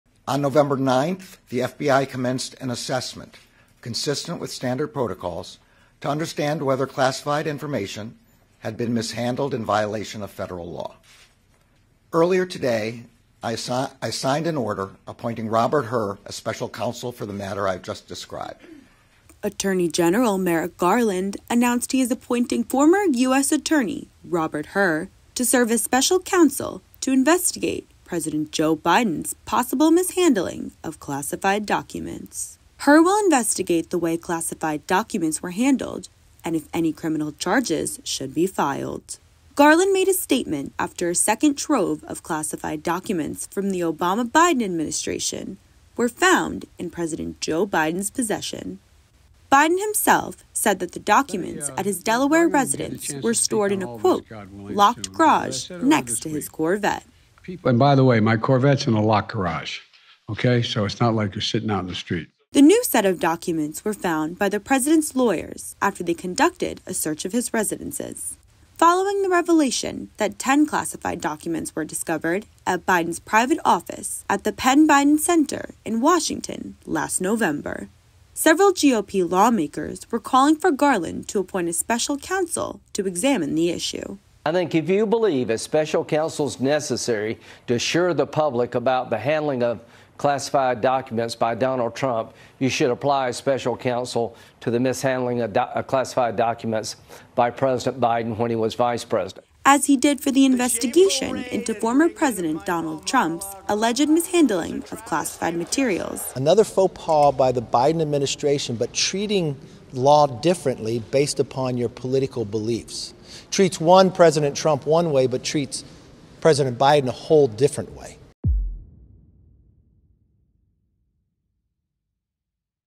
Garland appoints Robert Hur as special counsel to investigate classified Biden documents
Attorney General Merrick Garland announced Thursday he is appointing former U.S. Attorney Robert Hur to serve as special counsel to investigate President Joe Biden's mishandling of classified documents.

Hur will investigate the way classified documents were handled and if any criminal charges should be filed against Biden.